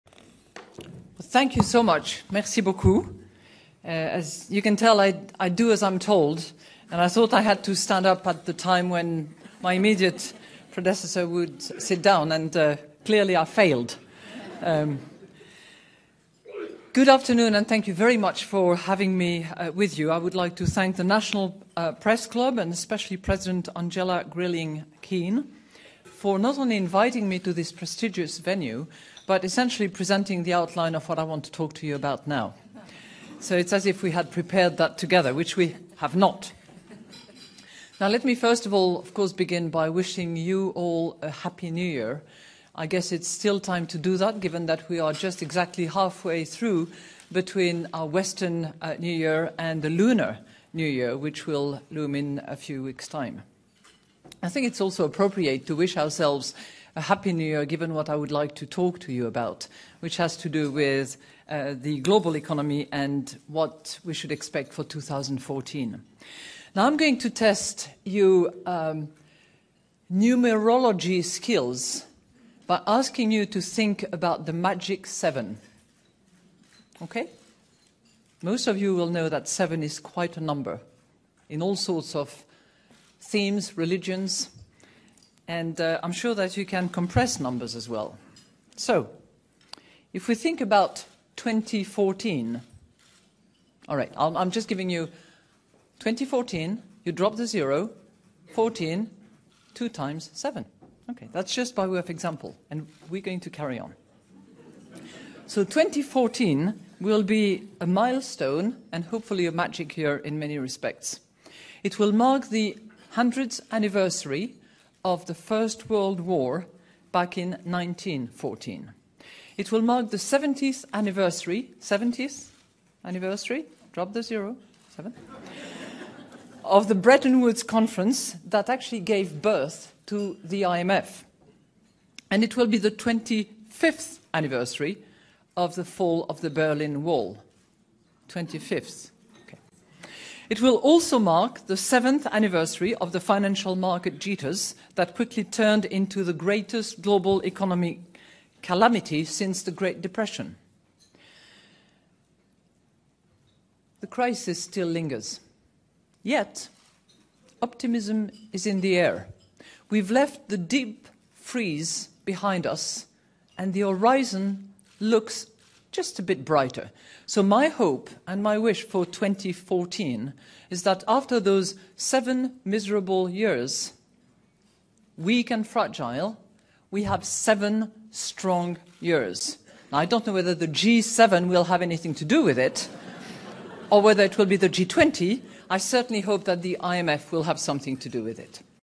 Managing Director of IMF Occult Message 777
Watch this video and remember that G is the 7th letter of the Alphabet so G7 can be thought of as 77.

I don't know the point of this strange video but If they are calling for a date to do something,  I predict Sept 14 is the day they are calling for here.

September was the 7th month in the Roman calendar, and 14=7+7. It is the 257th day of the year, 2+5=7+7=14 and there are 108 days left until the end of the year. She makes a point of referring to the lunar new year and the moon is 1080 miles in diameter (drop the  zero like she suggests).

It is 13 years and 3 days after September 11 where we can recall that flight 77 performed a 330 deg turn, dropping 2200 feet before crashing into the Pentagon.